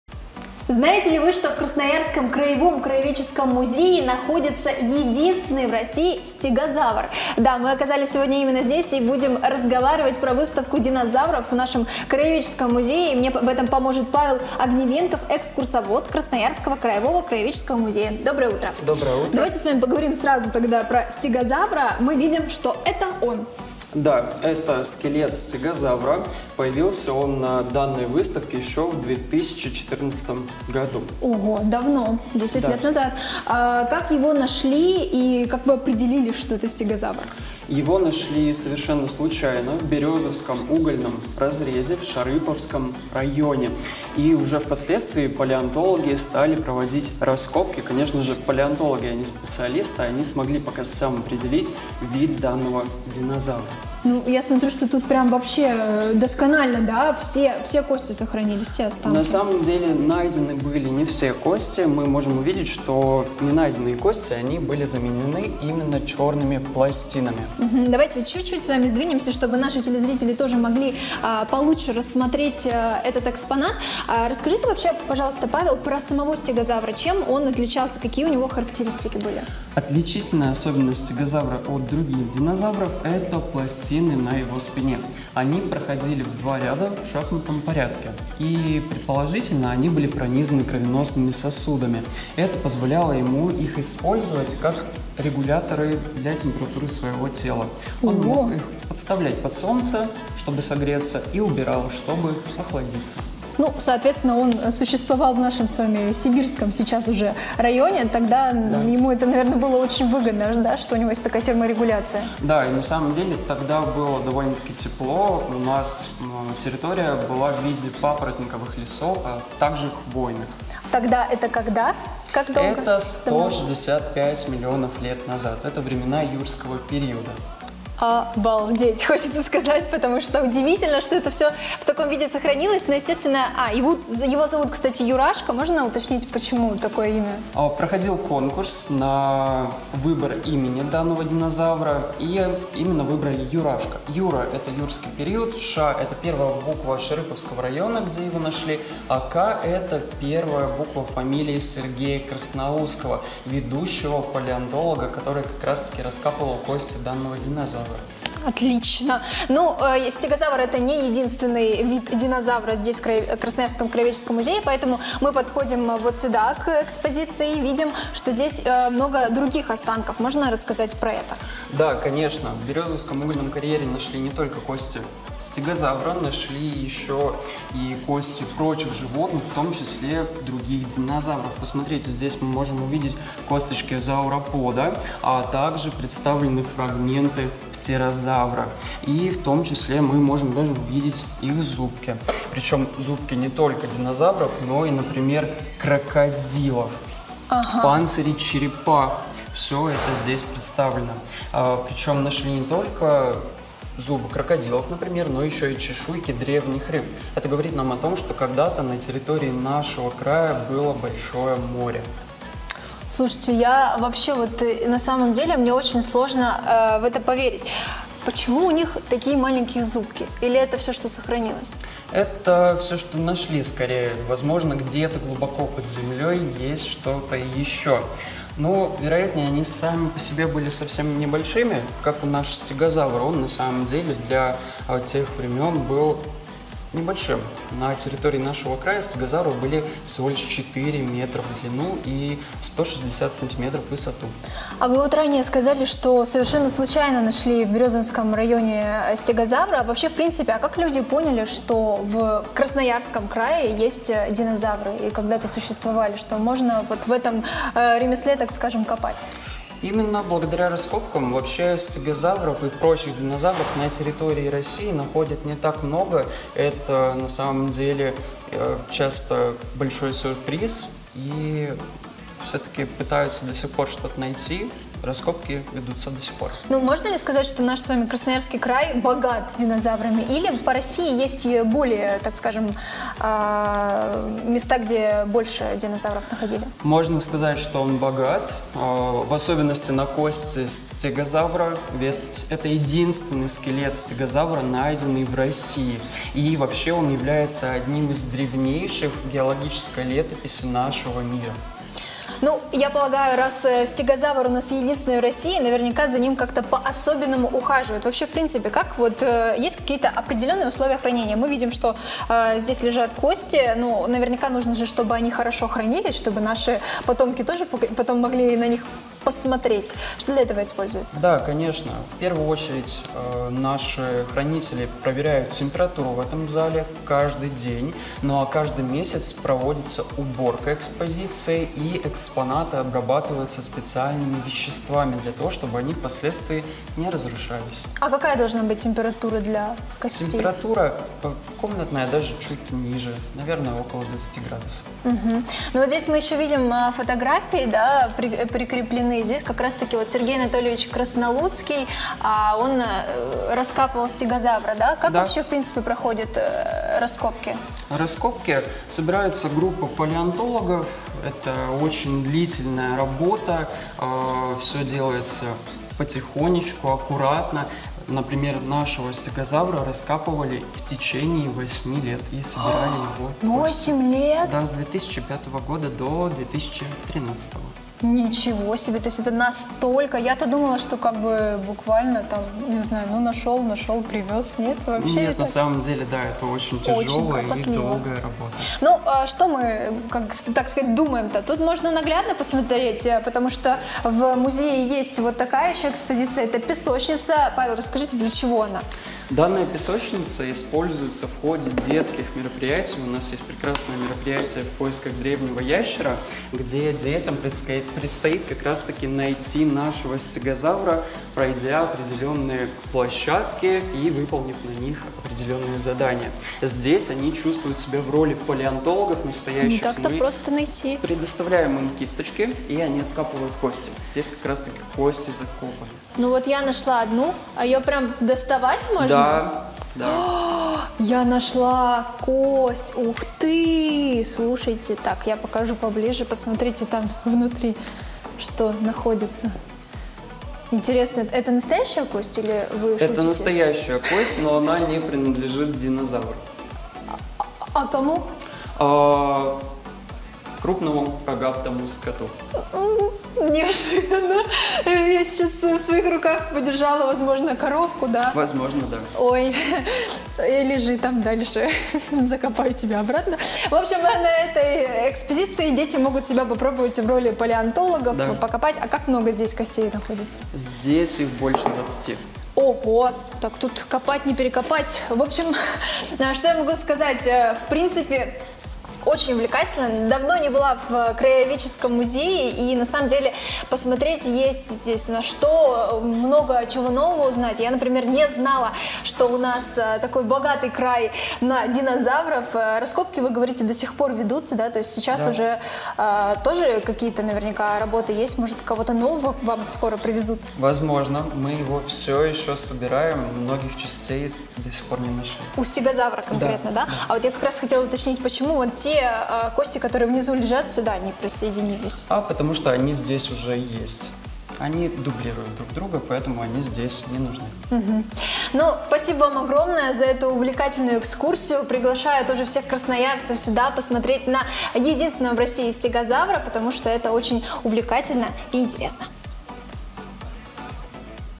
Выставка динозавров в Красноярском краеведческом музее
Какие экспонаты представлены на выставке? В чем их уникальность? Как эти экспонаты попали в музей? И как в музее ухаживают за ними? Об этом и не только рассказал экскурсовод Красноярского краевого краеведческого музея.